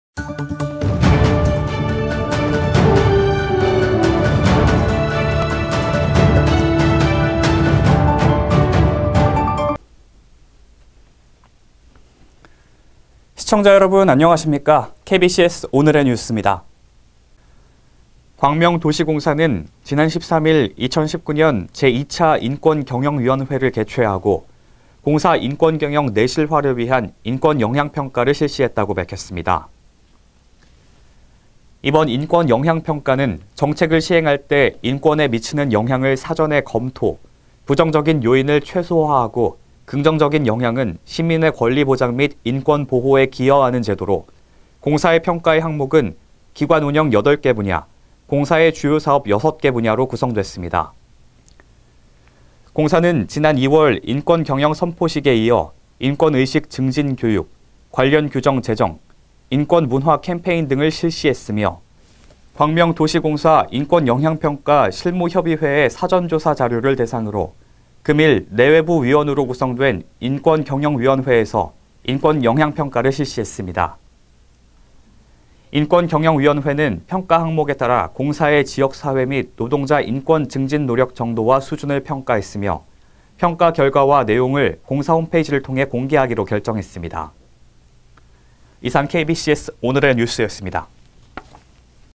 광명도시공사, 인권영향평가 실시
광명도시공사는 지난 13일(금) ‘2019년 제2차 인권경영위원회’를 개최하고 공사 인권경영 내실화를 위한 인권영향평가를 실시했다고 밝혔다.

이번 인권 영향평가는 정책을 시행할 때 인권에 미치는 영향을 사전에 검토, 부정적인 요인을 최소화하고 긍정적인 영향은 시민의 권리보장 및 인권 보호에 기여하는 제도로, 공사의 평가의 항목은 기관운영 8개 분야, 공사의 주요사업 6개 분야로 구성되었다.

공사는 지난 2월 인권경영선포식에 이어 인권의식 증진 교육, 관련 규정 제정, 인권문화캠페인 등을 실시했으며, 광명도시공사 인권영향평가 실무협의회의 사전 조사 자료를 대상으로 금일 내·외부 위원으로 구성된 인권경영위원회에서 인권 영향 평가를 실시했다.

인권경영위원회는 평가항목에 따라 공사의 지역사회 및 노동자 인권 증진 노력 정도와 수준을 평가했으며, 평가 결과와 내용을 공사 홈페이지를 통해 공개하기로 결정했다.